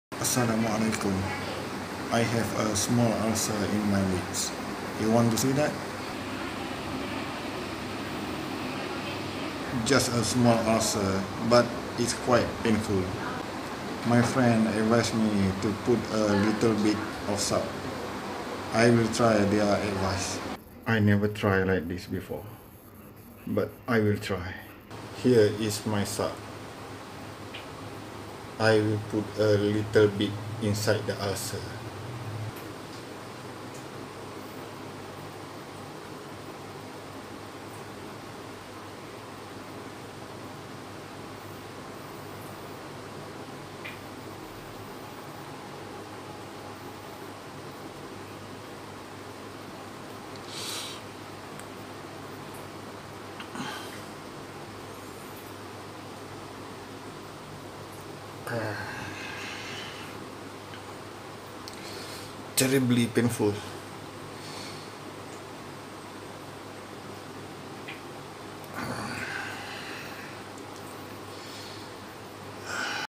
Ulser & salt
Ulser mulut juga dikenali sebagai serawan, guam, atau “mata ikan” ini adalah luka pada mukosa mulut. Biasanya terjadi pada bahagian dalam bibir, dalam pipi dan juga di permukaan lidah. Lukanya kecil, tapi ia sangat pedih.